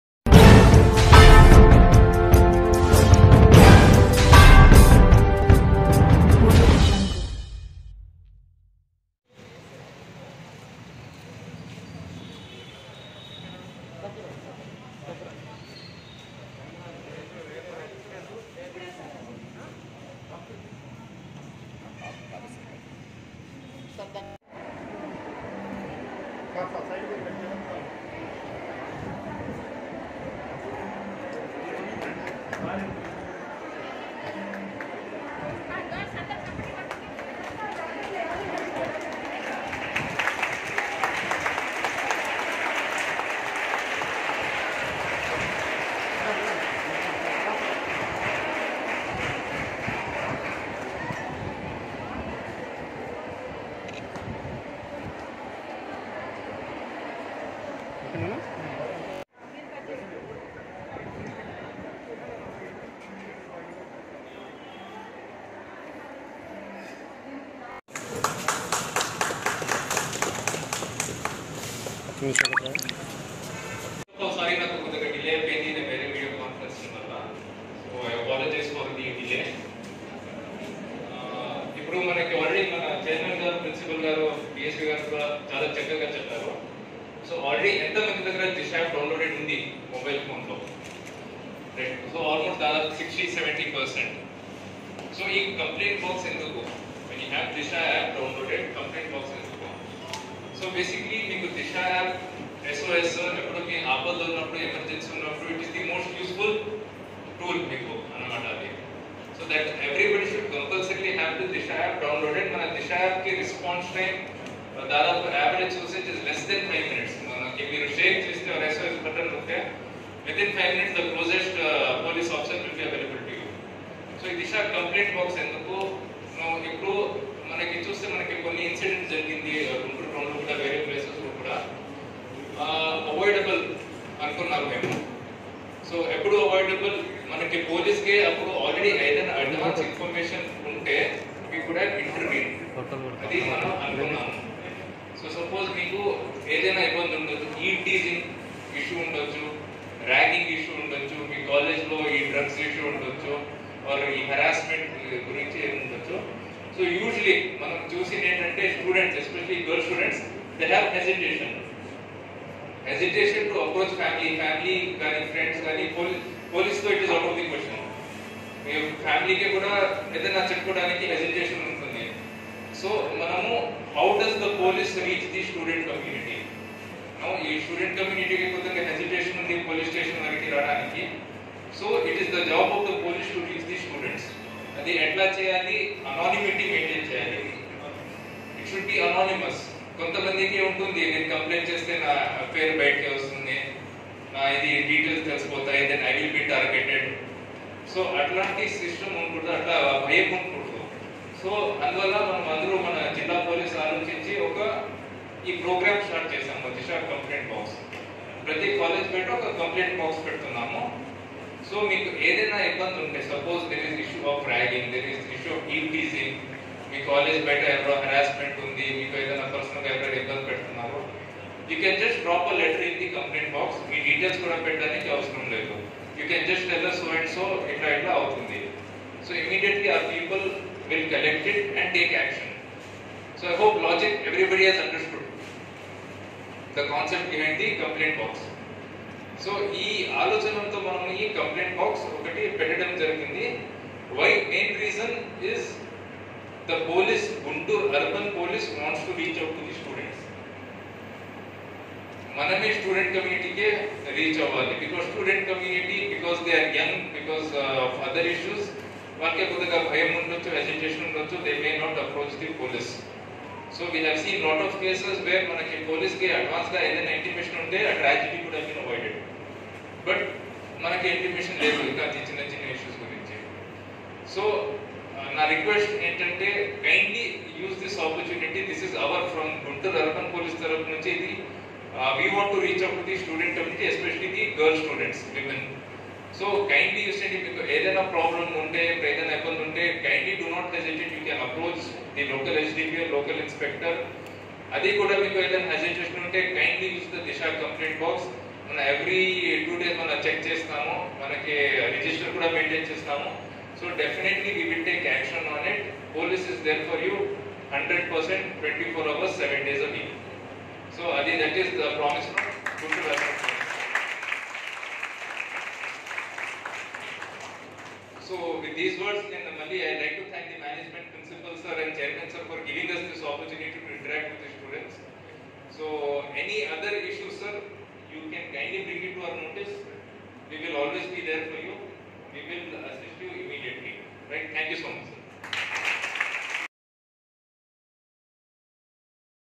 గుంటూరు లో" దిశ "ఫిర్యాదుల బాక్స్ లను ప్రారంభించిన అర్బన్ ఎస్పీ శ్రీ కె ఆరిఫ్ హఫీజ్ ఐపీఎస్..
హిందూ కాలేజీ ,గుంటూరు...